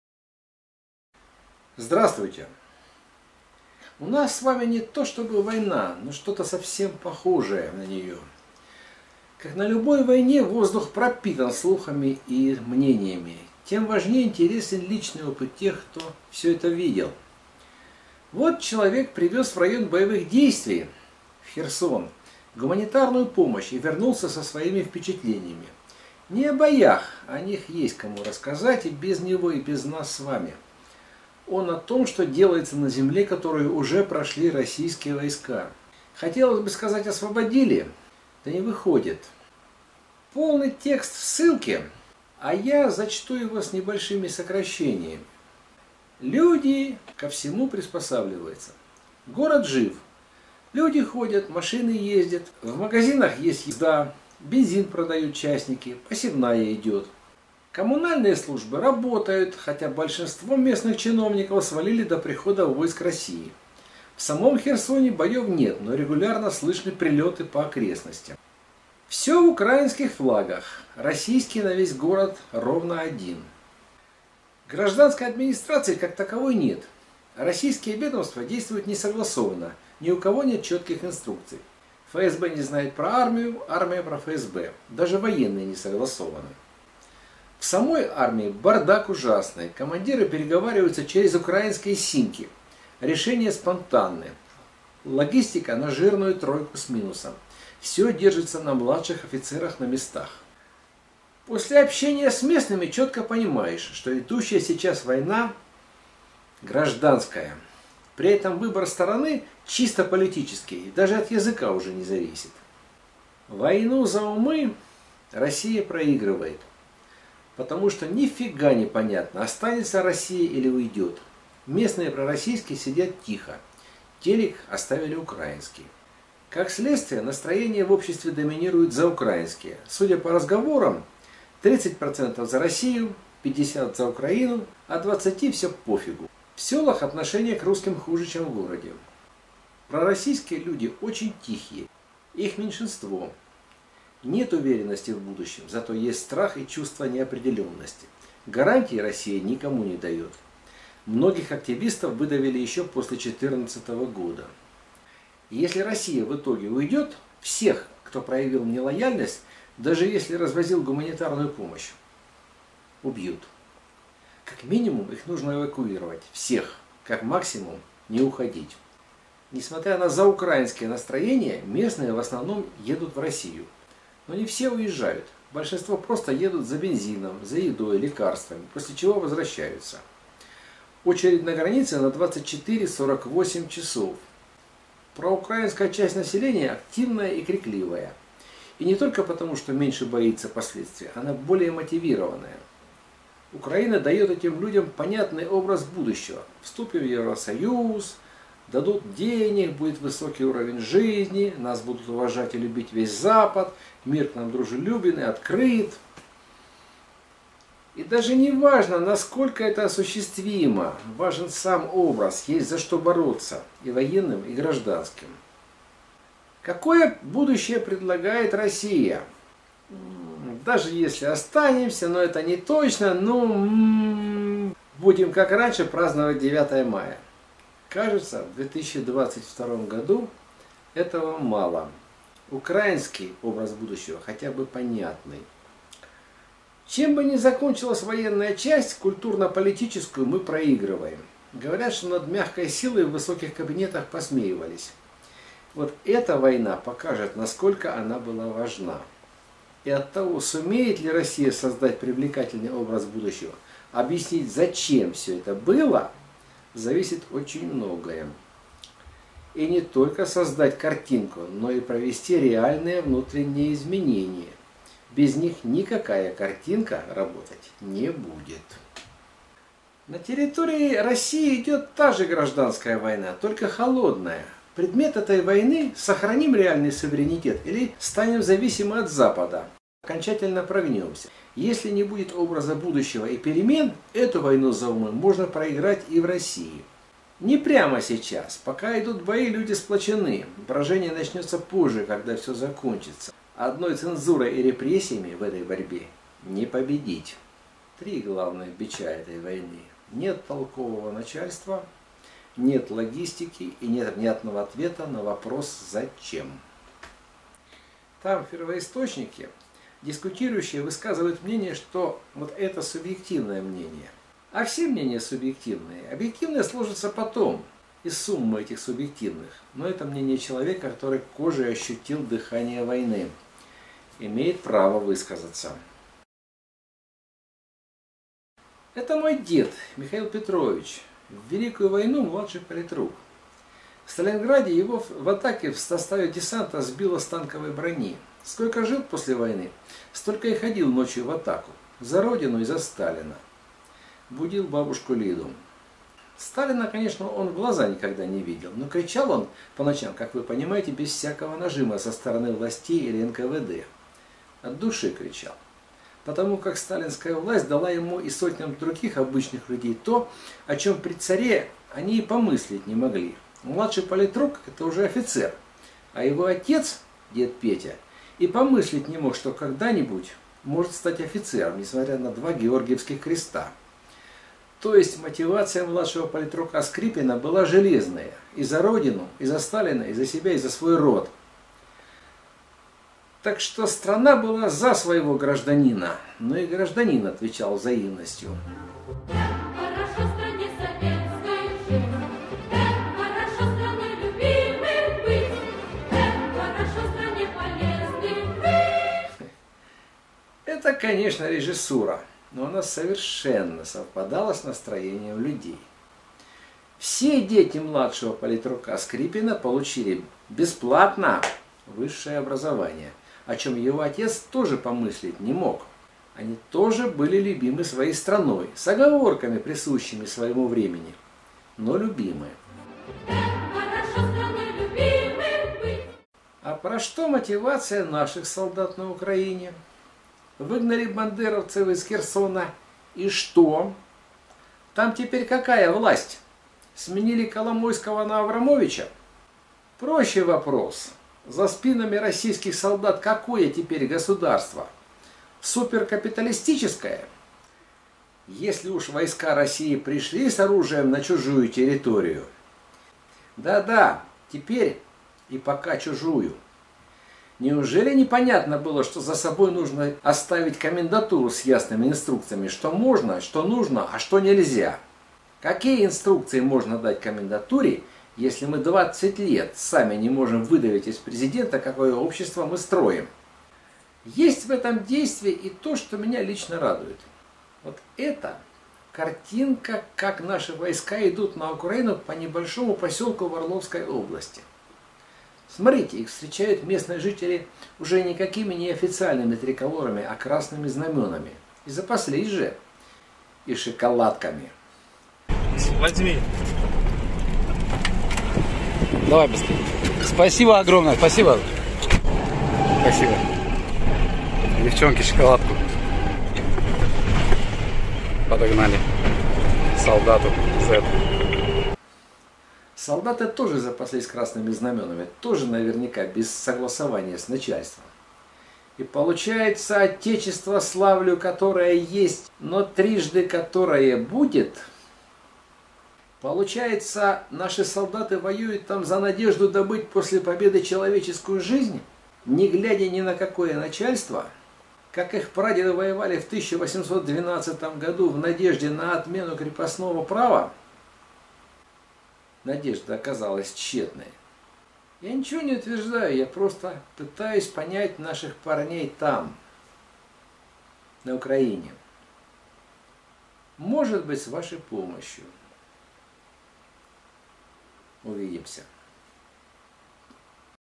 Возле войны 2
У нас с вами такая не война, что многие – а особенно начальство - не знают зачем это всё и что с этим делать... А многие от народа не знают, но чувствуют и надеются. Об этом ролик.